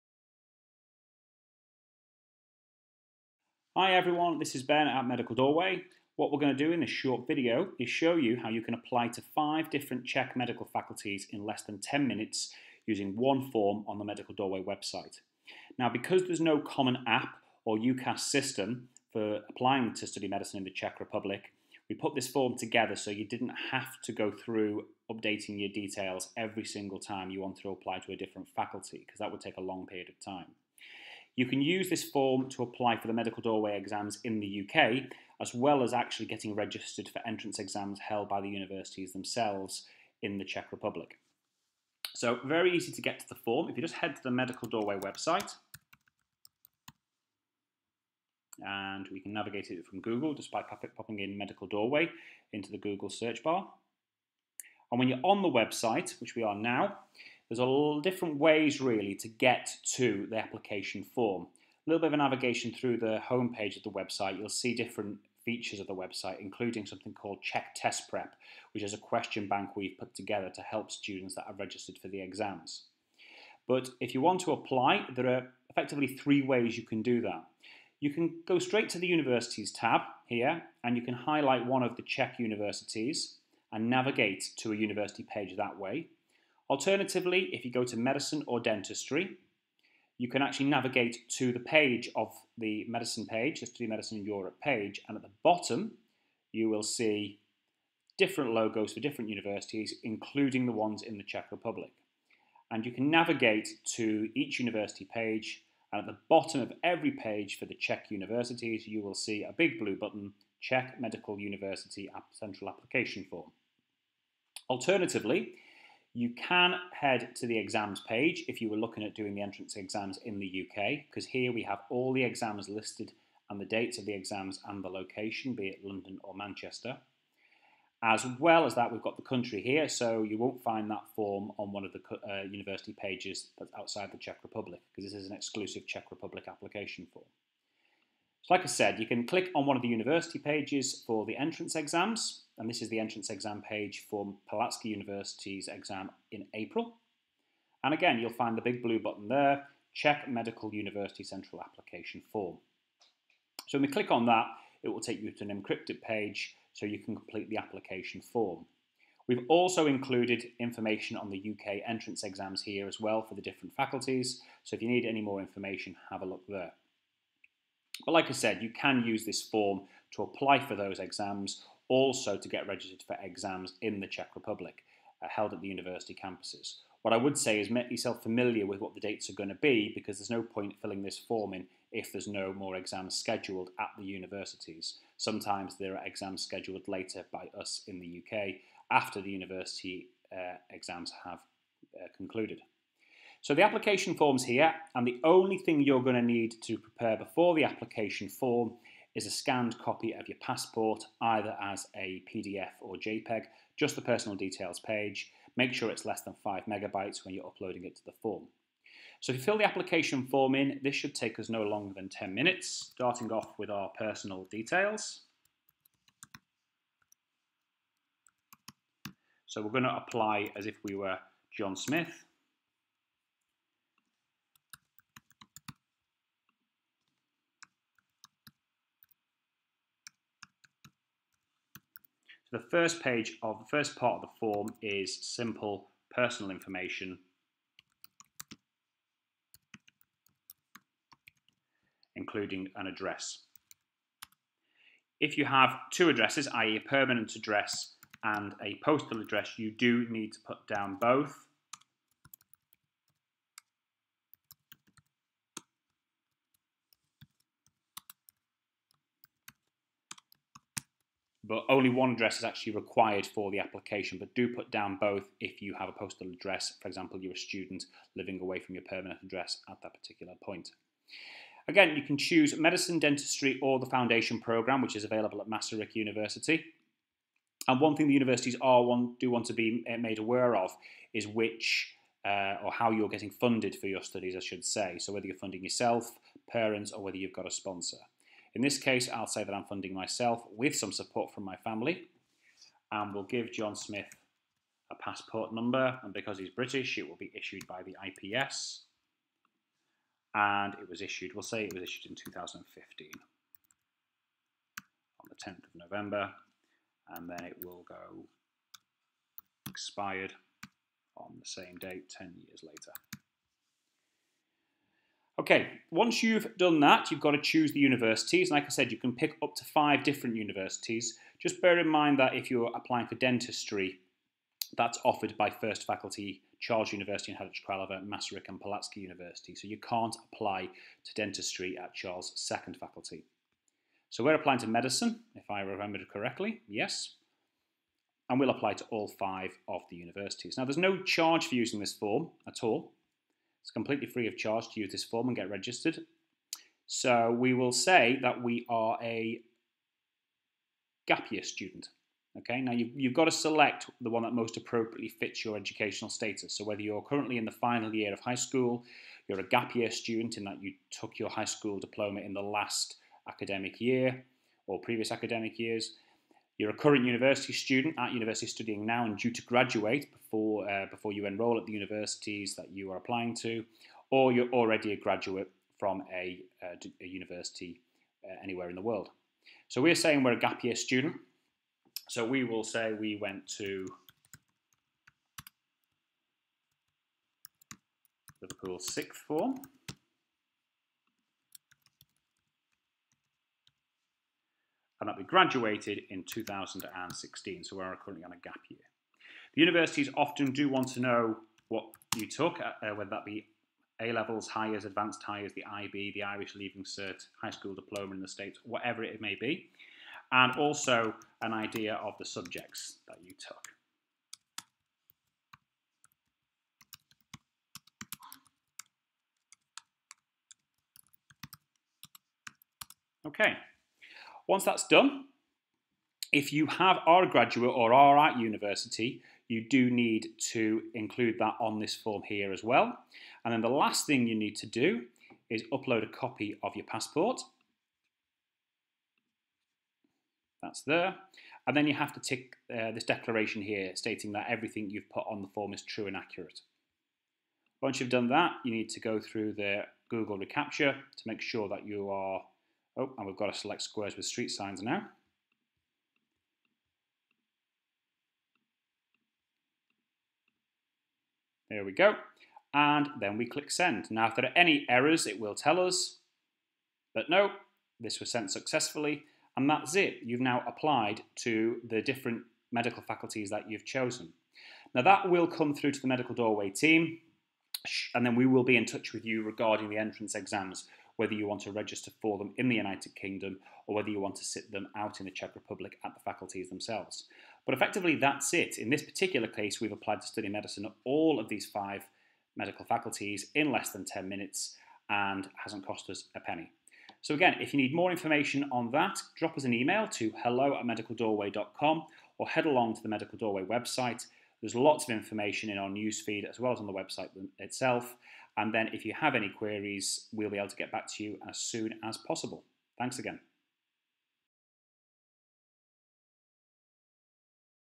Study Medicine in Czech Republic - Application Guide
Using Medical Doorway's "Czech Medical Central Application Form" you can apply for up to five Czech medical faculties in less than ten minutes.